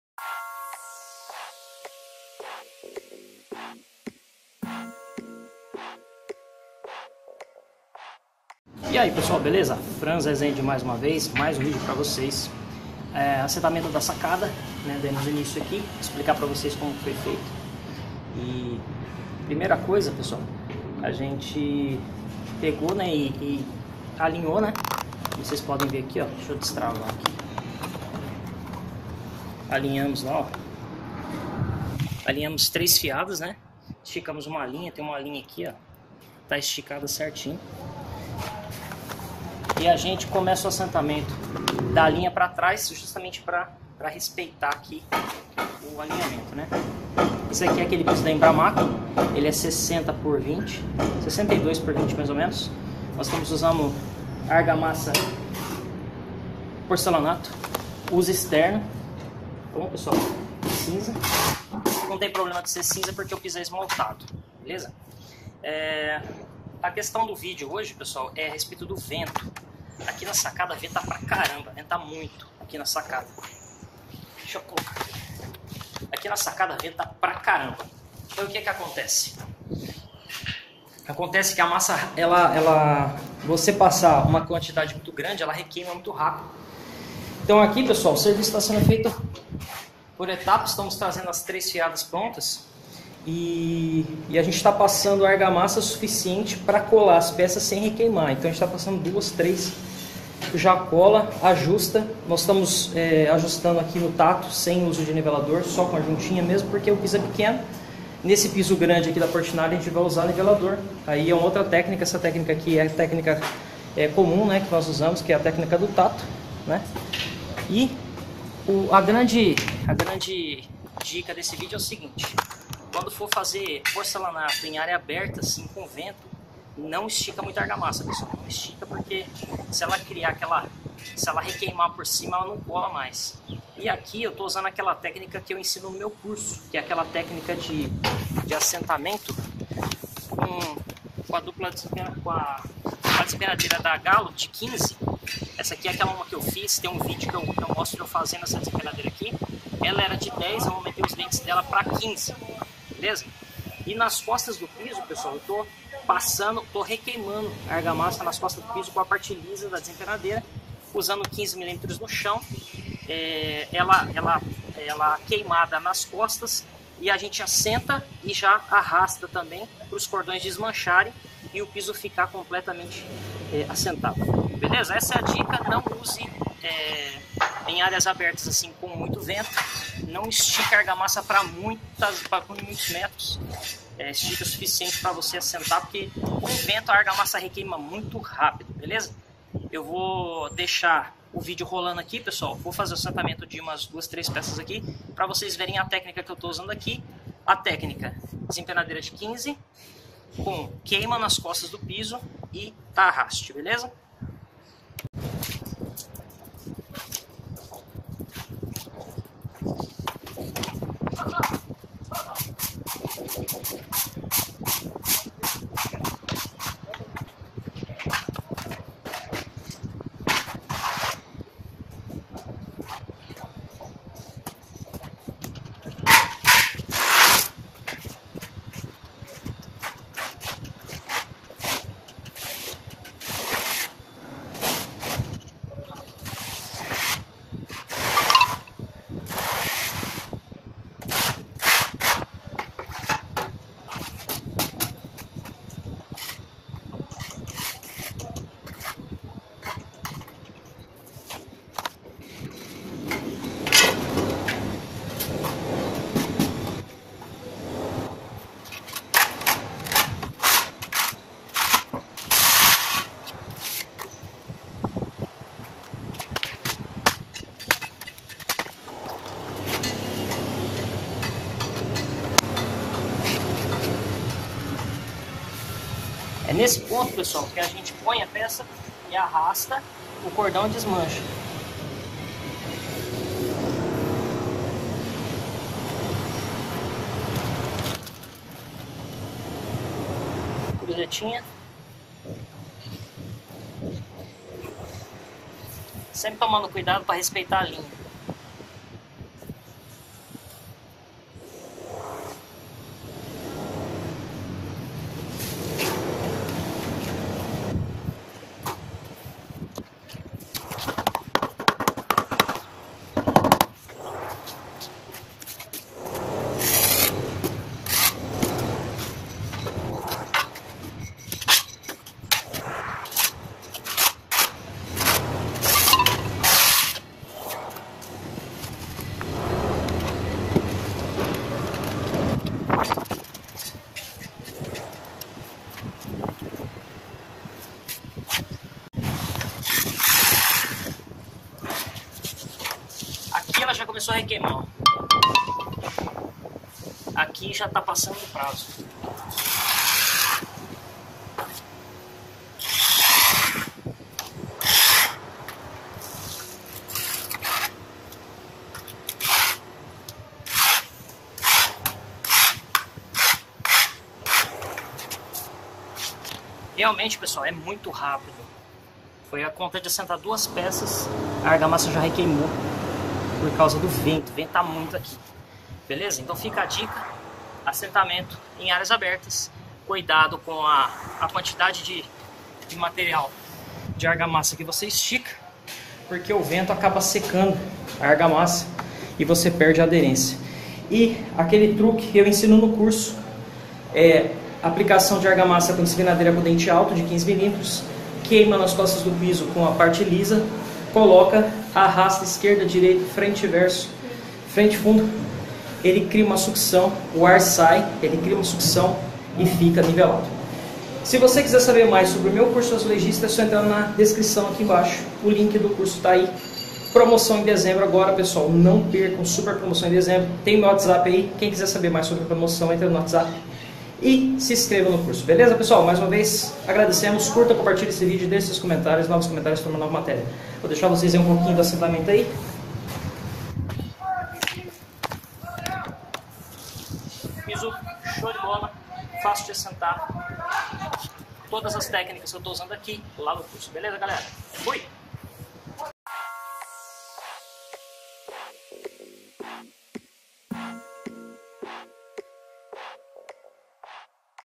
Sacada do apartamento.Como colocar piso na sacada sem soltar!Cuidado com esse inimigo.
Essa é a tecnica que eu ensino no curso O Azulejista
A tecnica de colocação de piso com queima e arraste é infalivel.
Ela aumenta a aderência da argamassa no piso,preenchendo todo o piso evitando pontos ocos.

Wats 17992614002